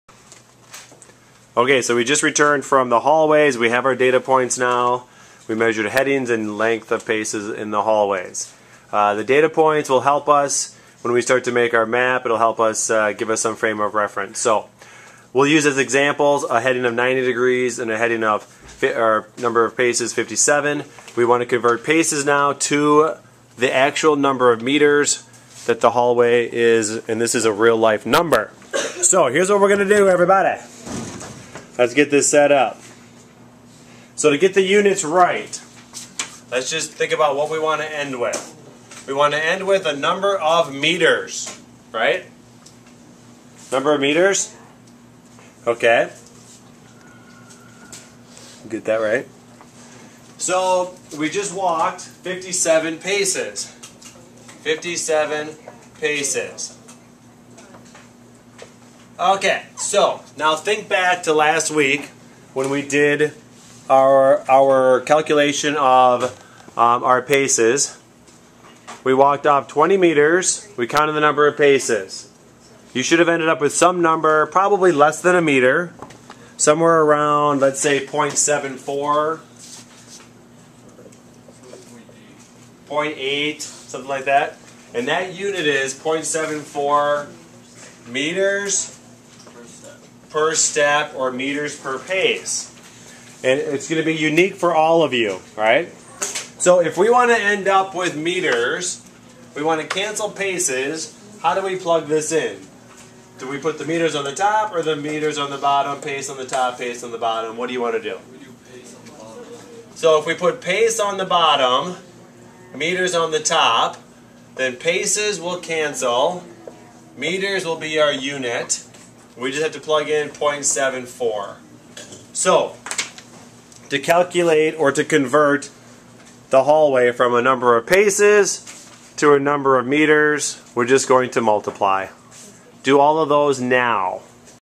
converting paces to meters
This video is part of our map making tutorials.  In this video Mr. Tiller goes through an example of how to convert a number of paces to meters in our map making process.